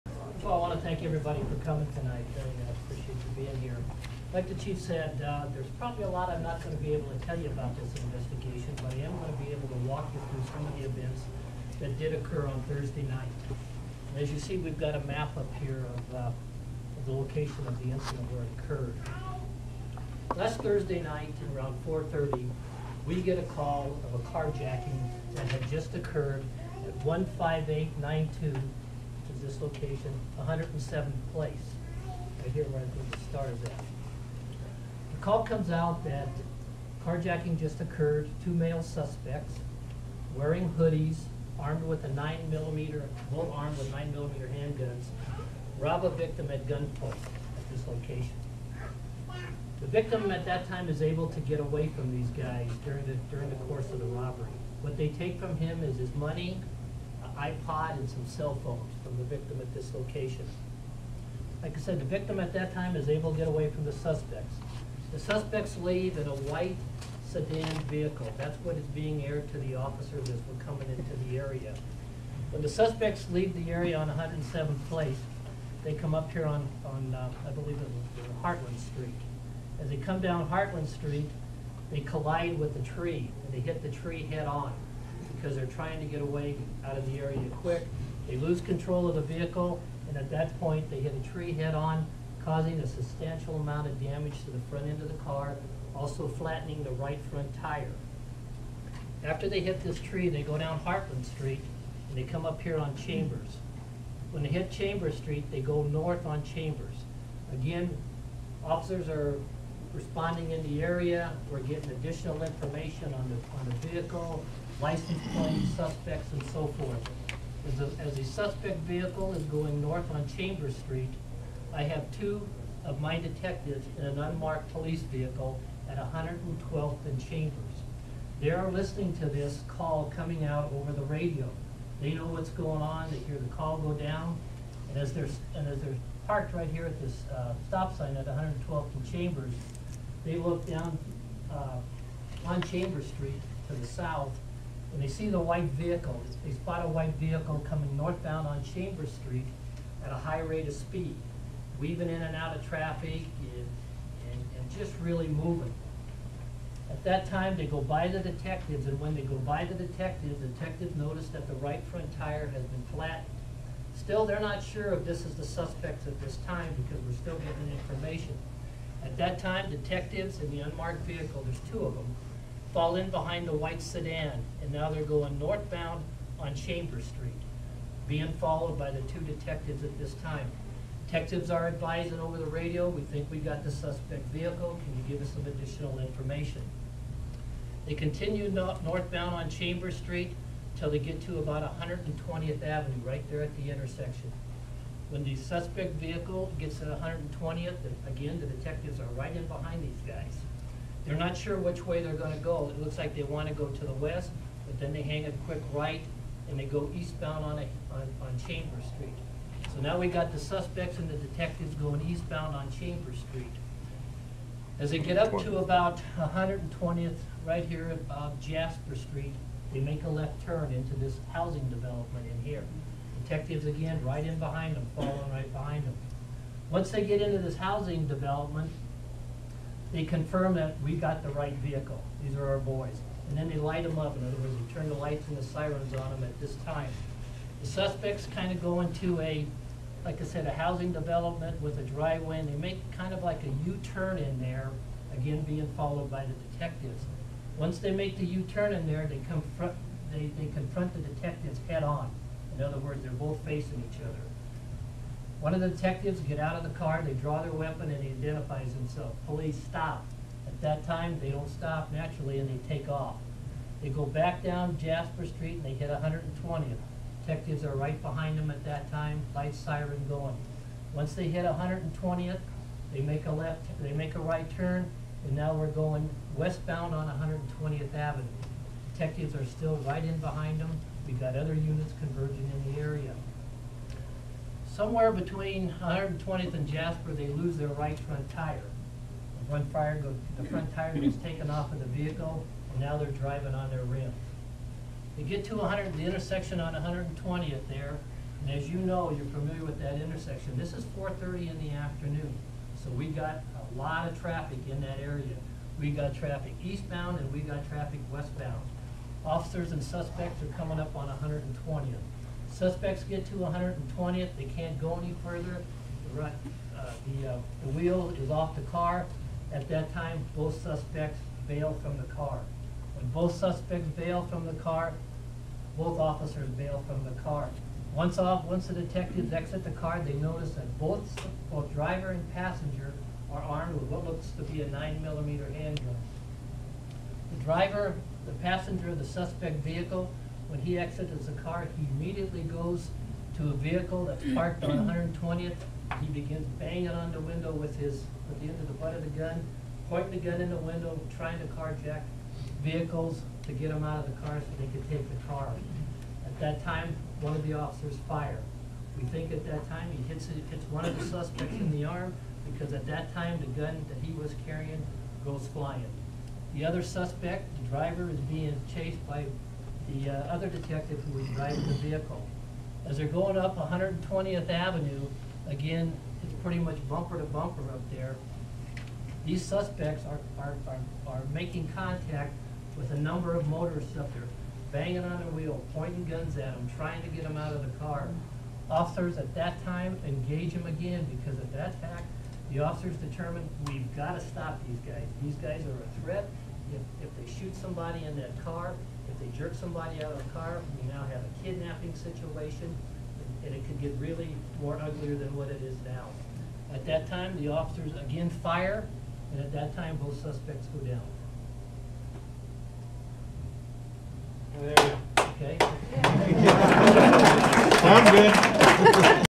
Commerce City Police Explain Events of Shooting at Reunion Meeting
At 3:59 p.m. Officers responded to a call of a Robbery and Car Jacking in the 15800 block of E 107th Ave. It was reported that two males had rear ended a car in front of them. When the car stopped they took the driver out and robbed him at gunpoint. The suspects then returned to their car and took off at a high rate of speed. As they were fleeing the area they lost control of their car at Jasper Street and Heartland, leaving the road and striking two trees and a street sign causing the right front tire to flatten. About this same time, two Commerce City Police Detectives were in the area on an unrelated investigation saw the suspects driving erratically. They attempted to stop the suspects but instead of stopping the suspects attempted to elude the officers. The suspects completely lost the right front tire of their vehicle at the intersection of E 120th Avenue and Chambers Road. The suspects exited their car and attempted to carjack other motorists at gun point. Detectives confronted the suspects and shots were fired resulting in both suspects being shot. The suspects then attempted to run from the scene but were caught just yards west of the intersection after a brief foot chase. Both suspects were transported to local hospitals where one of them later died from his injuries.

Both suspects are males in their early twenties names are not be released at this time for investigative reasons.

Adams County Critical Incident Team has been called out to investigate the shooting.